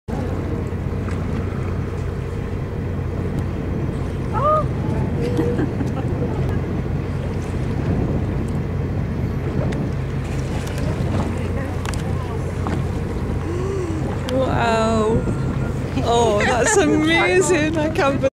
DolphinsAtKyleOfLochalsh.mpg
Wild dolphins in the Kyle of Lochalsh, Scotland.  They like to hang around one of the salmon farms.  Naturally curious and come right up to the tourist boat.